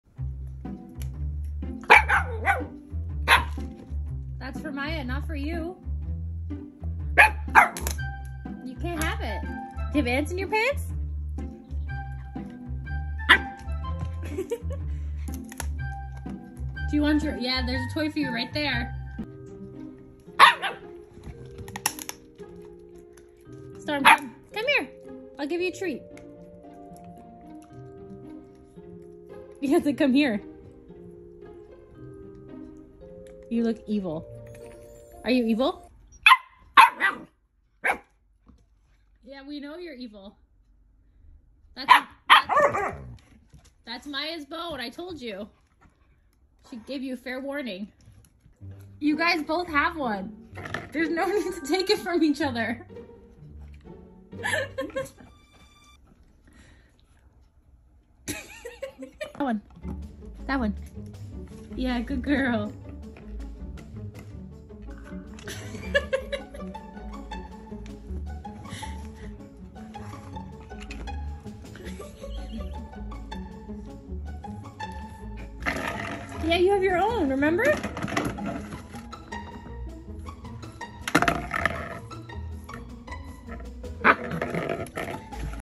Husky Puppy Argues to Get her Sister’s Bone!
Husky Puppy Tries to Steal Her Sisters Bone!

Storm was trying to steal her Sister Maya the Husky's bone!
Maya is VERY patient with Storm, but will correct her as needed.
The correction Maya gave her was not aggression, puppies learn from other dogs.

Storm is quite the chatty husky puppy, sometimes even leaving Maya to be the quiet husky in the house!